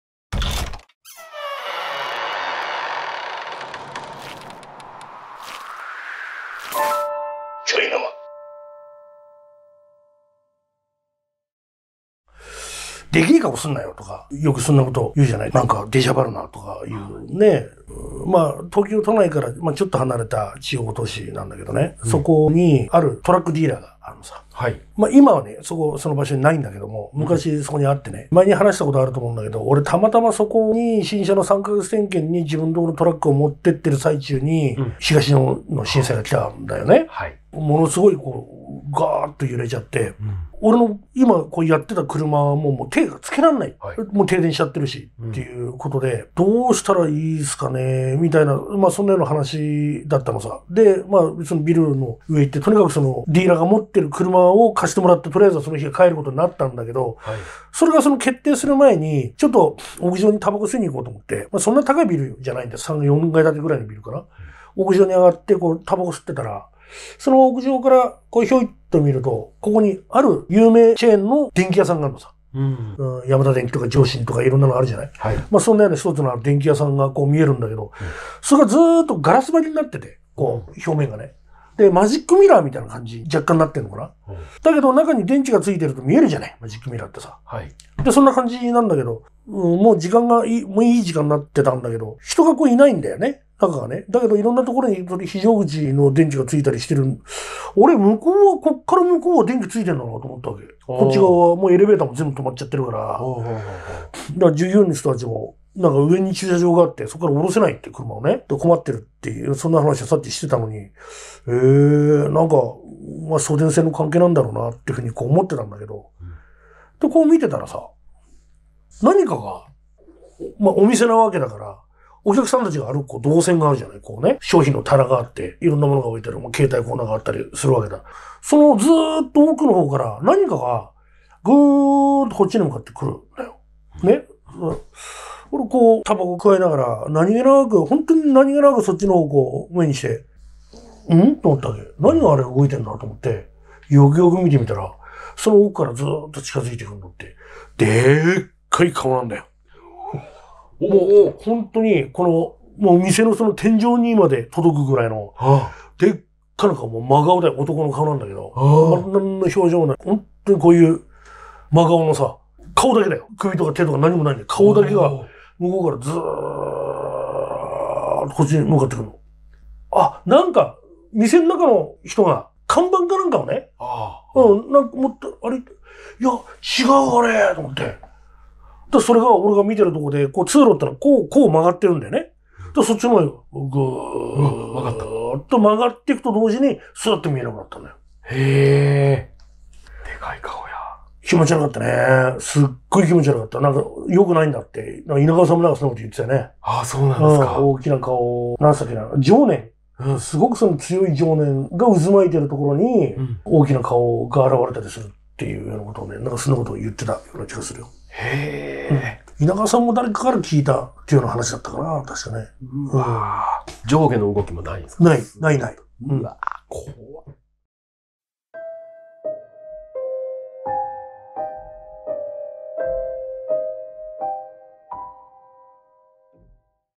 怪談:ちょいの魔57話【大きな顔】ファンキー・中村とパウチが放つ怪談&バラエティ。2023年最新版です！
最恐怪談【魔守りの虎】フルバージョンがみられるメンバーシップはこちら
ファンキー·中村の豊富な心霊実体験をパウチが聴き編集してお届け！
●GhostContents公式グッズ発売中
●個人チャンネル
●SNS他情報
　【ファンキー・中村】